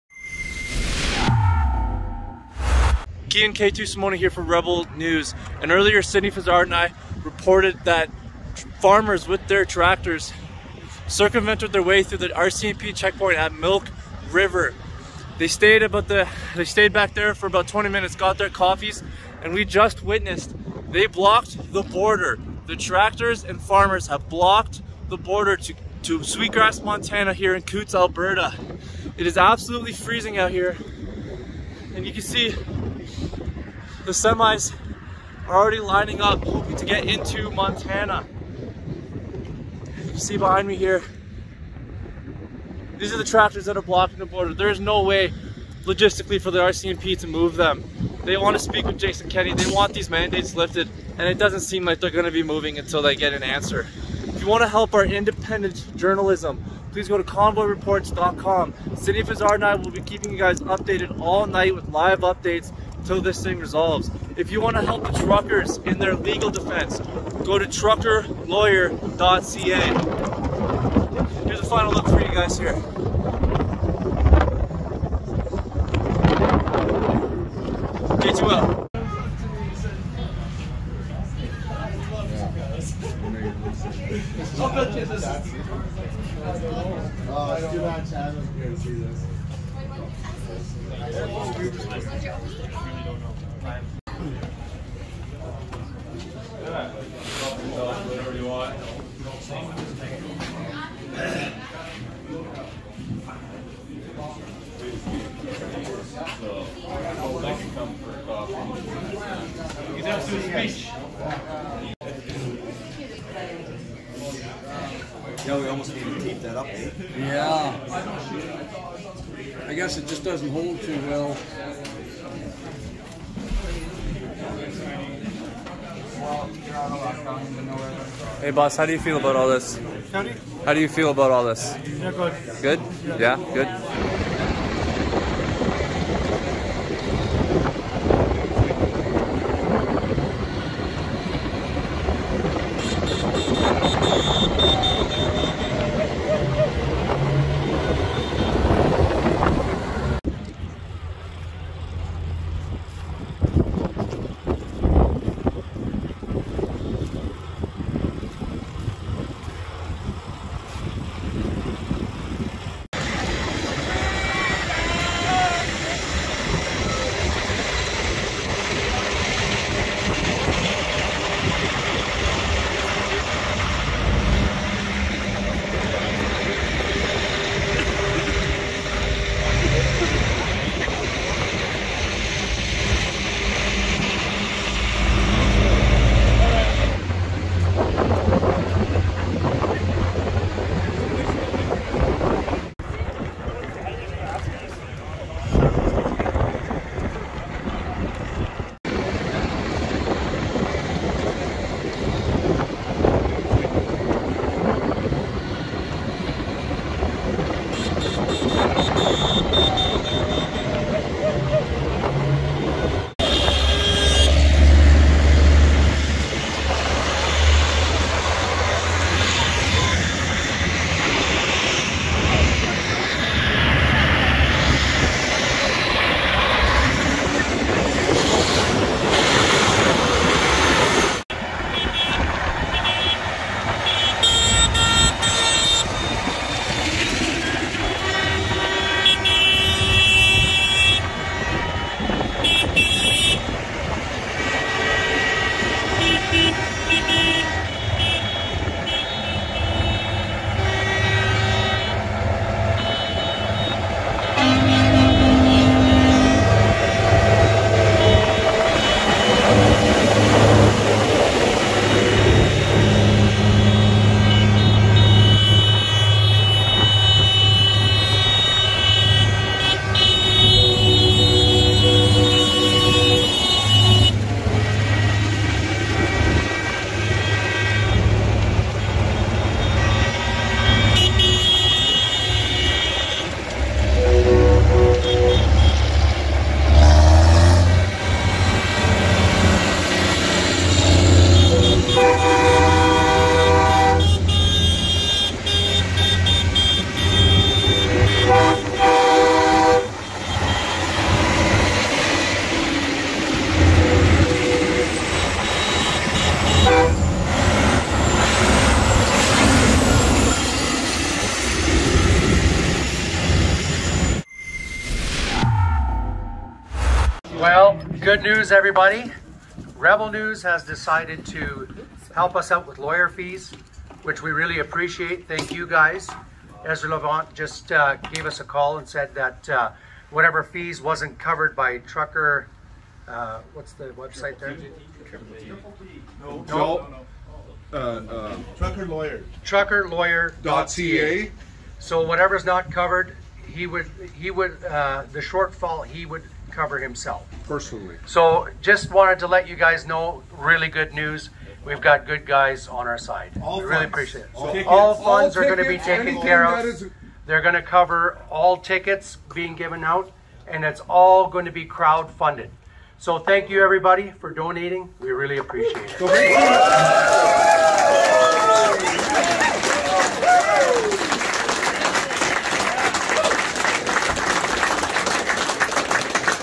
Coutts border crossing attracts 10,000 truckers on weekend
Last Saturday roughly 10,000 truckers gathered to participate in a slow roll at the Coutts/Sweet Grass border in protest of the mandates.